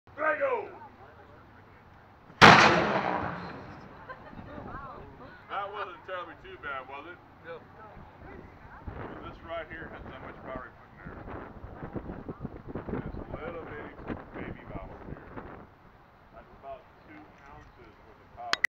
Cannon Firing (St. Augustine The Fountain Of Youth Park)
Hello friends, it been a crazy long time since I've uploaded, three months right? Anyway, I hope you read my reminder in my channel's discussion because I and my family took a trip to Florida for Spring Break! How exciting! Anyway, at this cool park place called TFOYP there was cannon firing show so I hope you enjoy. Warning, it is loud... At least for me...